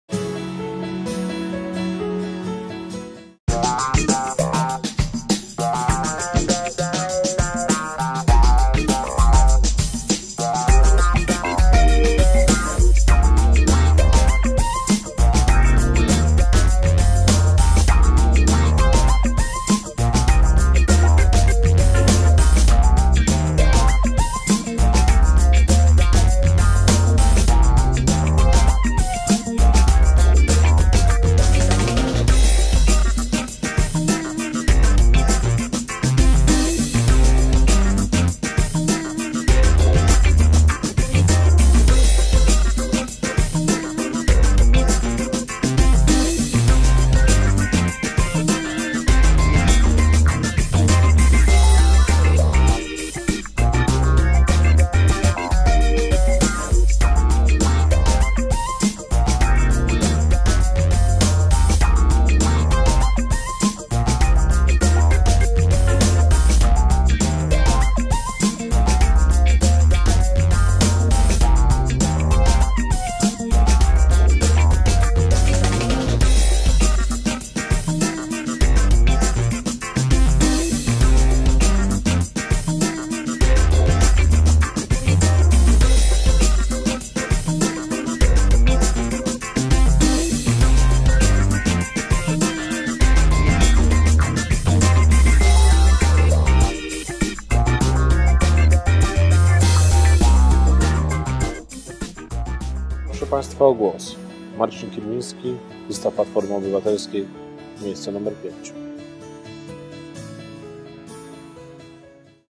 Konwencja Programowa PO na Ursynowie
Konwencja programowa na Ursynowie, oficjalna prezentacja programu wyborczego Platformy Obywatelskiej na wybory 2011.